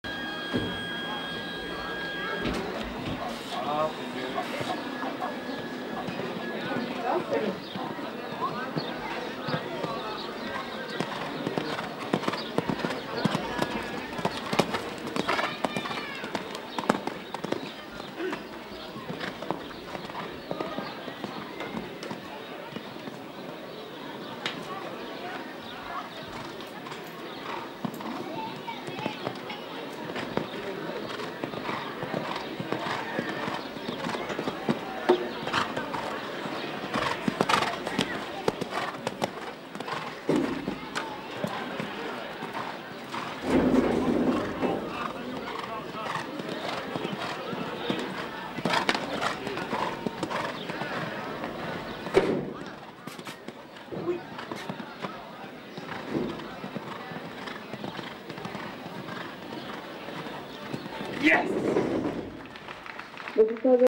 Kabili - Prova Grande - 1.35m - Badajoz
Kabili is 13 yo glelding Selle Francais by Qredo de Polstra